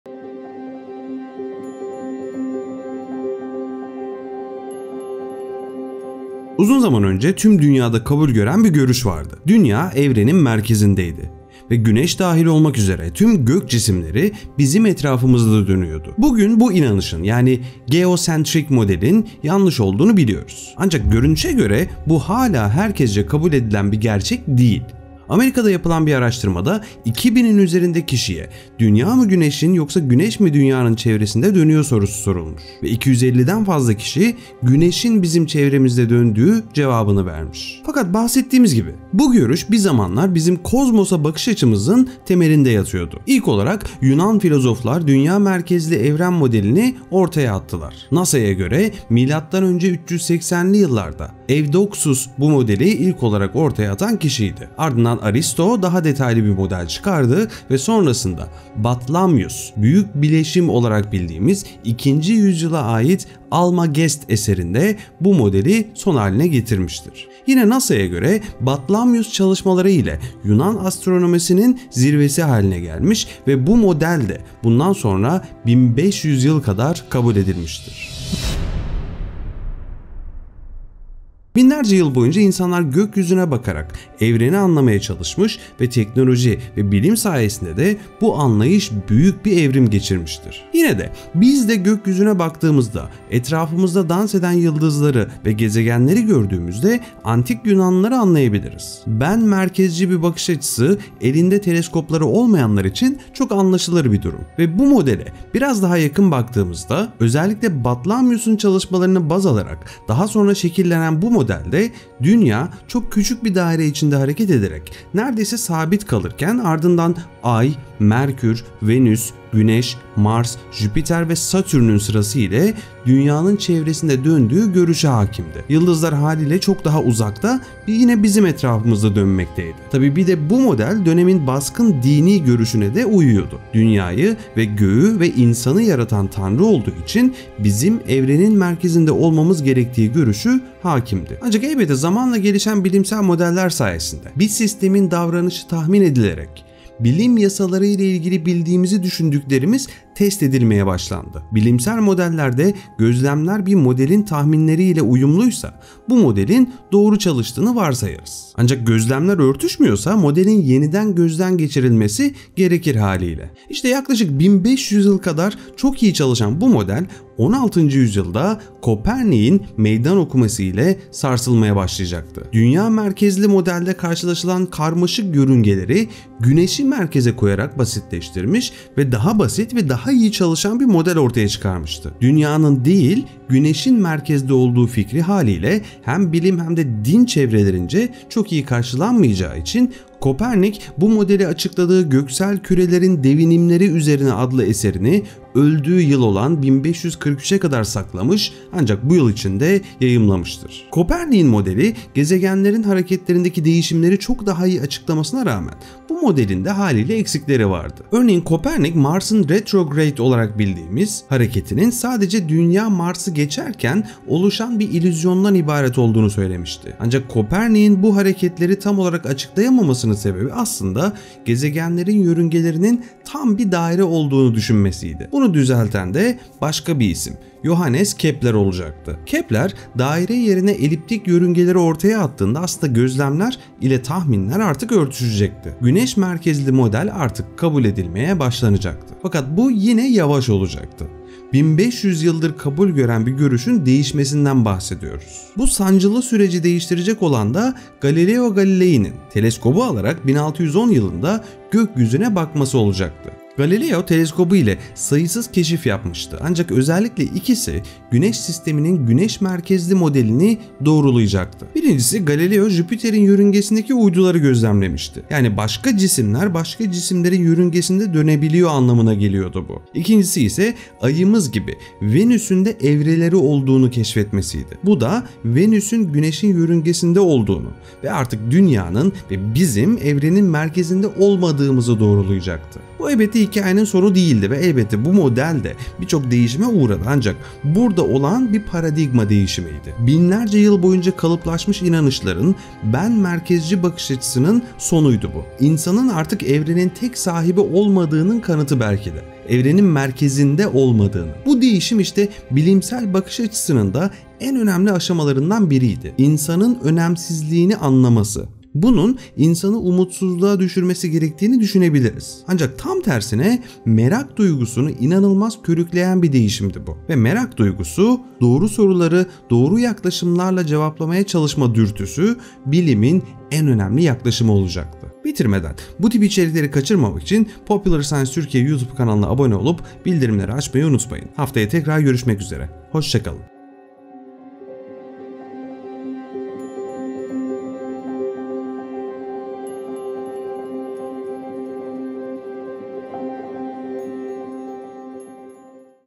1500 Yıllık Yanlış: Evrenin Merkezi Neresi? | Popular Science Türkiye
Uzun zaman önce tüm dünyada kabul gören bir görüş vardı. Dünya evrenin merkezindeydi ve güneş dahil olmak üzere tüm gök cisimleri bizim etrafımızda dönüyordu. Bugün bu inanışın, yani “geocentric” modelin yanlış olduğunu biliyoruz ancak görünüşe göre bu hala herkesçe kabul edilen bir “gerçek” değil.

Amerika’da yapılan bir araştırmada 2000’in üzerinde kişiye “Dünya mı güneşin yoksa güneş mi dünyanın çevresinde dönüyor” sorusu sorulmuş ve 250’den fazla kişi güneşin bizim çevremizde döndüğü cevabını vermiş.

Popular Science'ı sosyal ağlarda takip etmek için tıklayın!

Bu kanal Popular Science Türkiye'nin resmi YouTube hesabıdır!